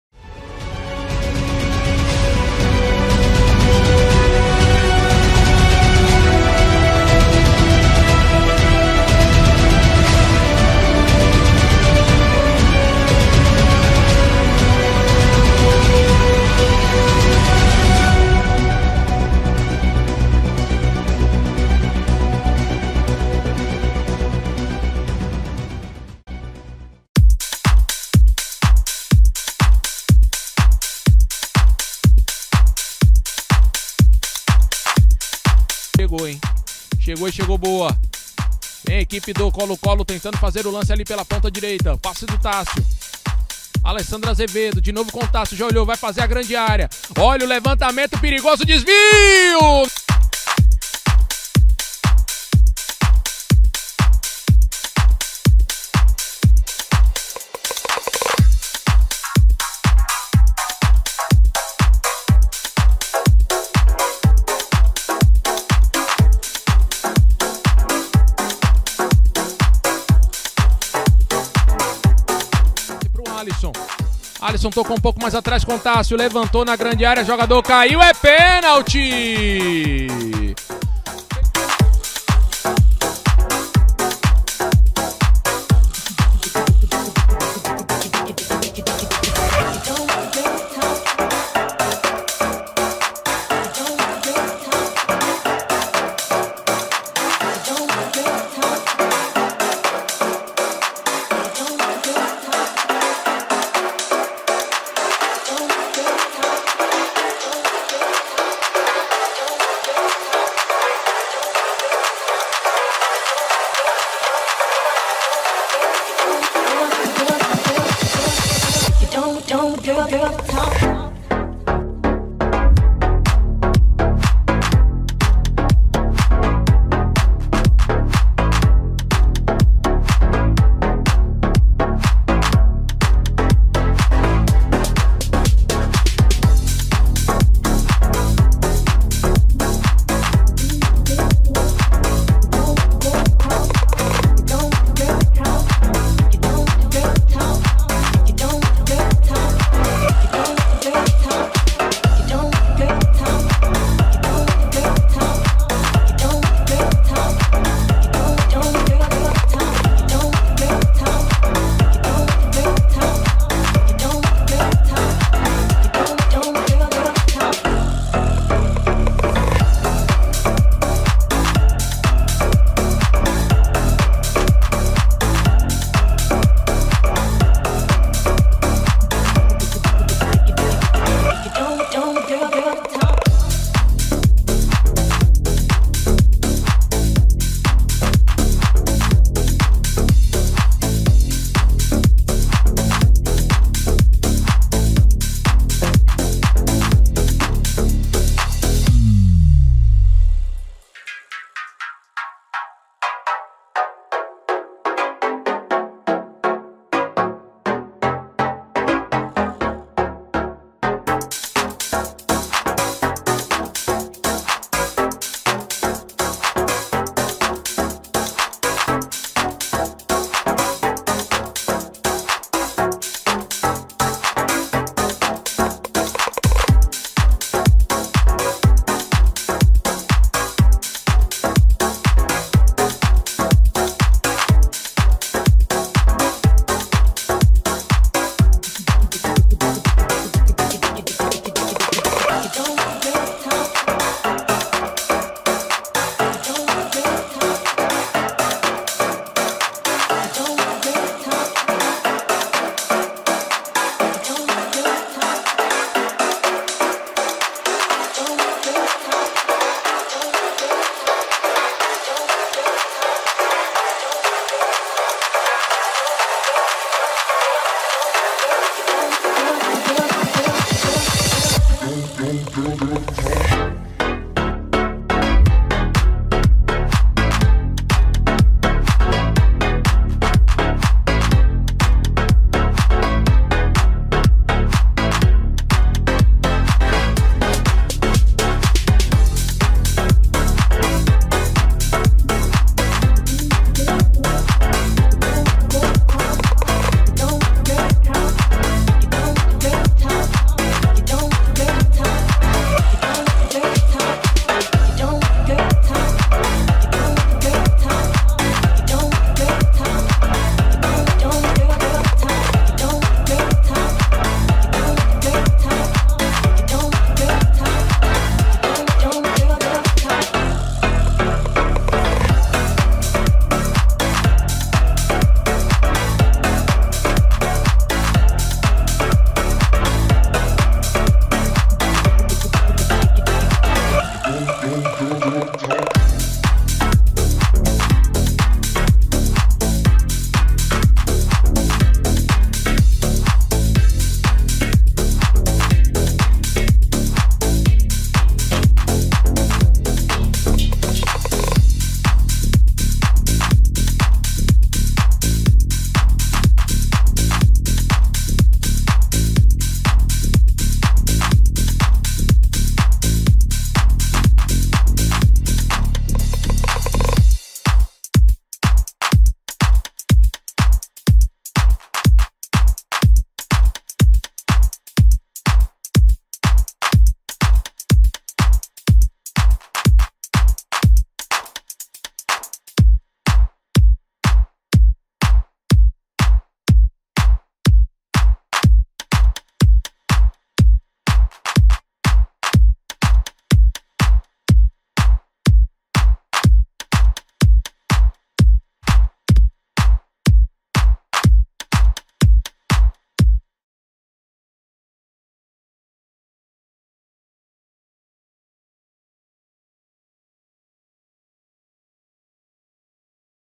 DVD - TÁSSIO NASCIMENTO | LATERAL-DIREITO/VOLANTE 2021
ATLETA PROFISSIONAL - PROFESSIONAL ATHLETE
NAME: TASSIO NASCIMENTO
BIRTHDATE: 05/07/1993
POSITION: RIGHT-SIDE/ MIDFIELDER
HEIGHT: 1,68m
WEIGHT: 61kg
FOOT DOMINANT: RIGHT
NATIONALITY: Brazilian